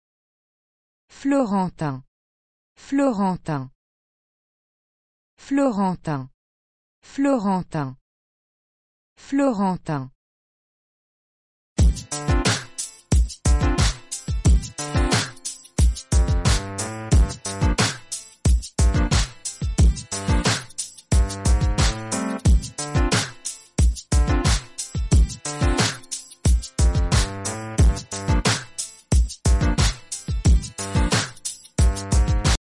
How do you say 'Florentine' in French?
How to say 'Florentine' in French? Listen to hear the pronunciation.